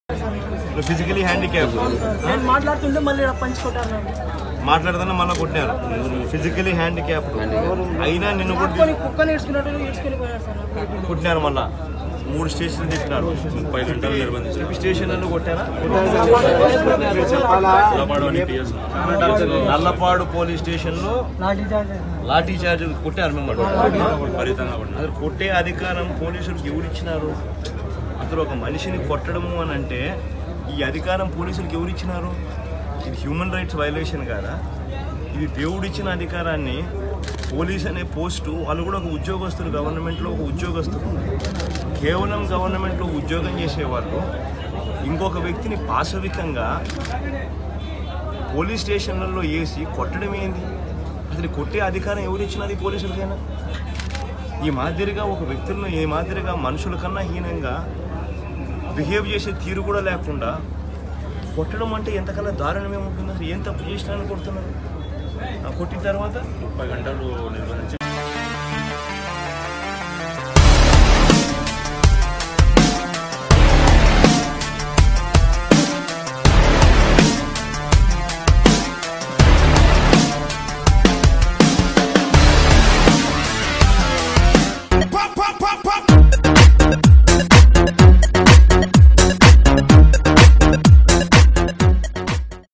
జగన్ ను కలిసి పోలీసు హింస వివరించిన ముస్లిం యువకులు| Muslim youths described police torture to Jagan
Muslim Youths, Who are arrested by Police at Chandrababu Naidu Guntur Nara Hamara Program meet YS Jagan. They Explained Jagan about Police Torture.